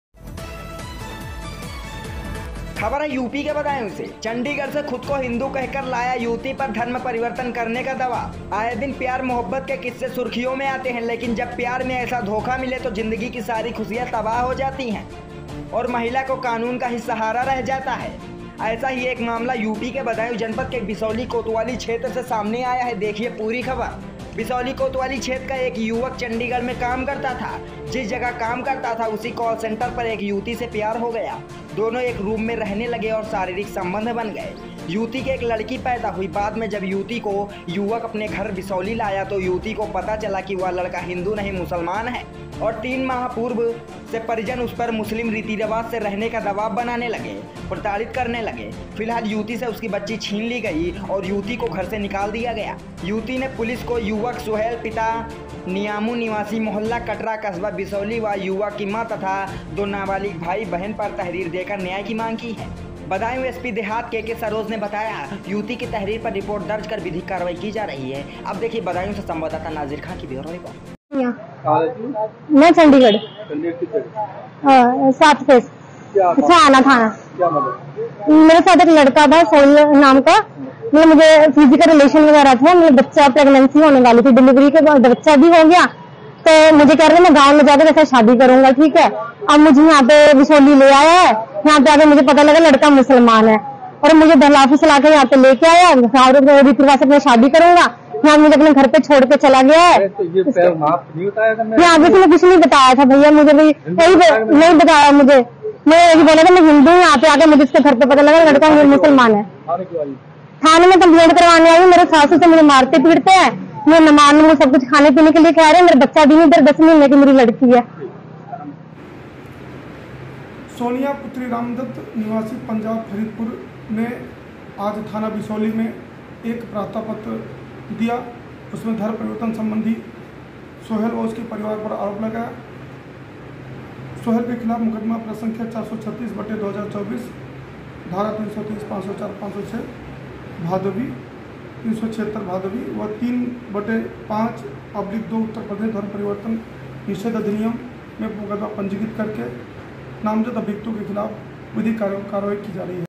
UP BARABANKI - चंडीगढ़ से खुद को हिन्दू कह कर लाया युवती पर धर्म परिवर्तन करने का दबाव
बदायूं

चंडीगढ़ से खुद को हिन्दू कह कर लाया युवती पर धर्म_परिवर्तन करने का दबाव

आए दिन प्यार मोहब्बत के किस्से सुर्खियों में आते हैं लेकिन जब प्यार में ऐसा धोखा मिले तो जिंदगी की सारी खुशियां तबाह हो जाती है और महिला को कानून ही सहारा रह जाता है ऐसा ही एक मामला बदायूं की बिसौली_कोतवाली क्षेत्र से सामने आया है देखिए पूरी खबर

बिसौली कोतवाली क्षेत्र का एक युवक चंडीगढ़ में काम करता था जिस जगह काम करता था उसी काल सेंटर पर एक युवती से प्यार हो गया दोनों एक रूम में रहने लगे शारीरिक संबंध बन गए युवती के एक लड़की पैदा हुई , बाद में जब युवती को युवक अपने घर बिसौली लाया तो युवती को पता चला कि वह मुसलमान है और तीन माह पूर्व से परिजन उस पर मुस्लिम रीति_रिवाज से रहने का दबाव बनाने लगे प्रताड़ित करने लगे, फिलहाल युवती से उसकी बच्ची छीन कर युवती को घर से निकाल दिया, युवती ने पुलिस को युवक सोहेल पिता नियामू निवासी मोहल्ला कटरा कस्बा बिसौली ब युवक की मां तथा दो नाबालिग भाई बहिन पर तहरीर देकर न्याय की मांग की है